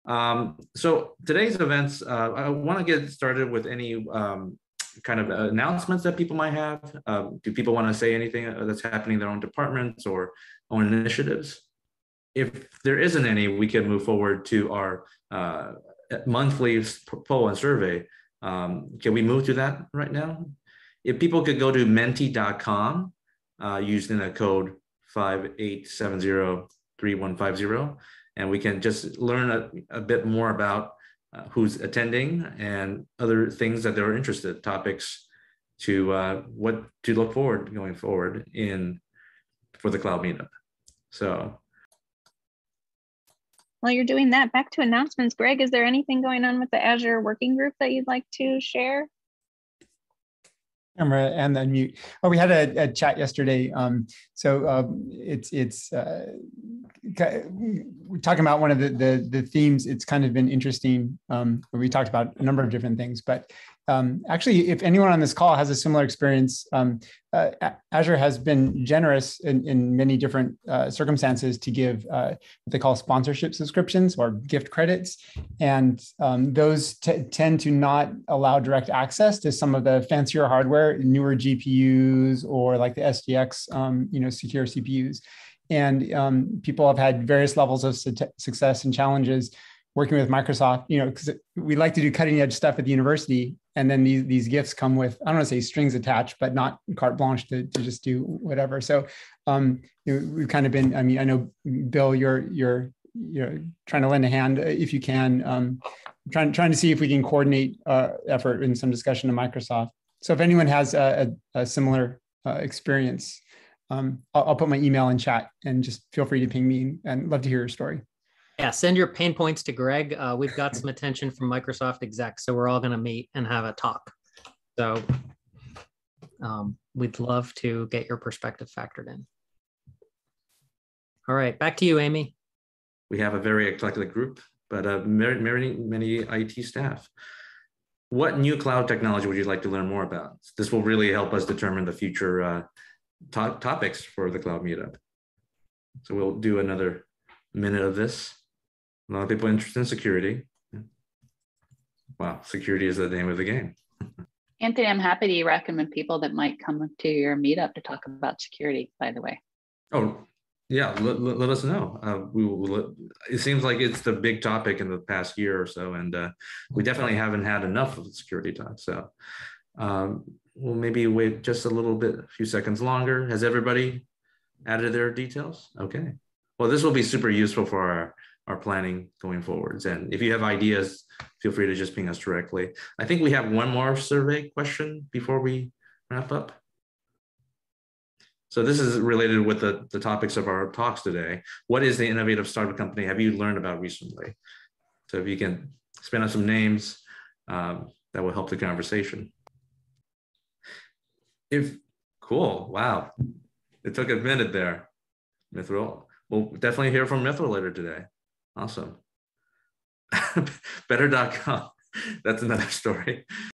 030-01 - Intro - UC Berkeley Cloud Meetup 030 (Dec. 9, 2021)
Welcome / announcements / poll (10 min) Host: Anthony Suen